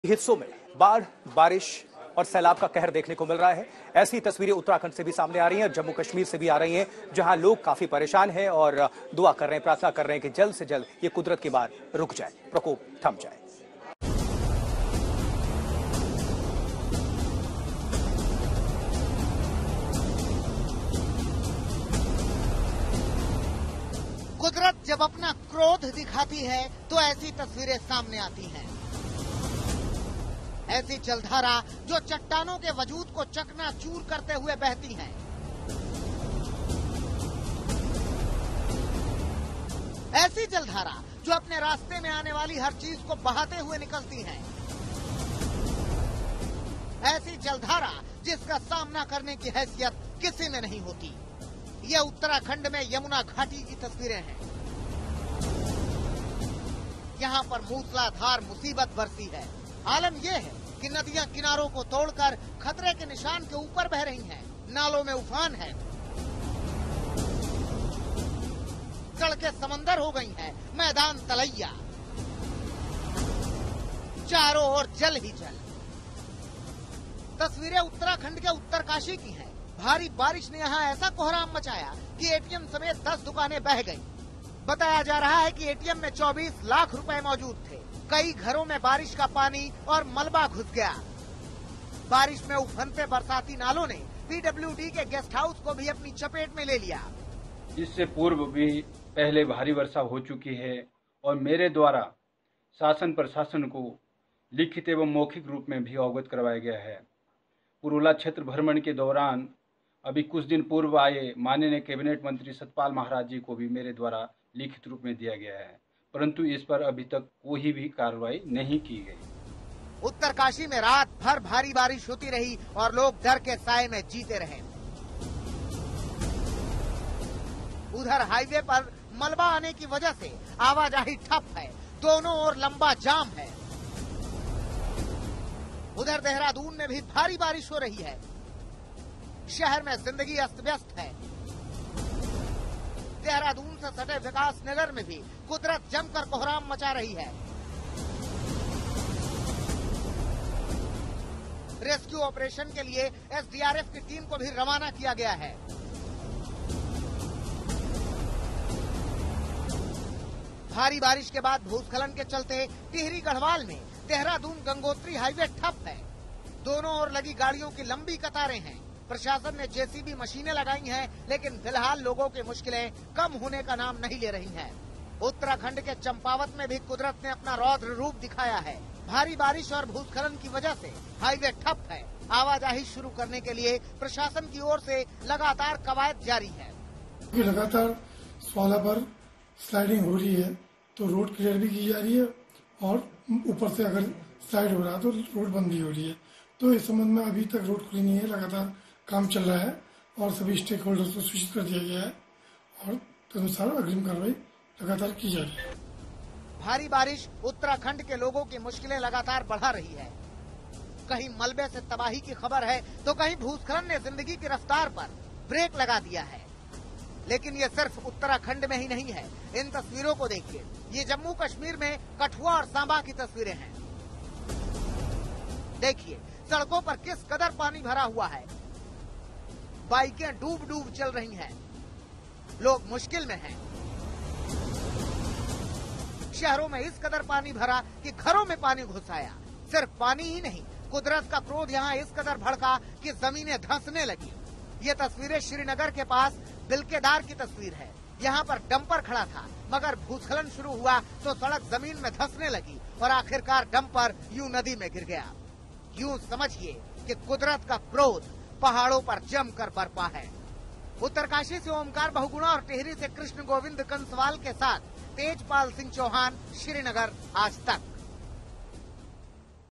Uttrakhand Flood: उत्तराखंड में बाढ़ बारिश का कहर, सैलाब में बह गए घर-गाड़ियां | Latest News
Nature's fury continues to wreak havoc in the hills. From Jammu and Kashmir to Himachal Pradesh multiple incidents of cloudbursts, flash floods and landslides have been reported in these states.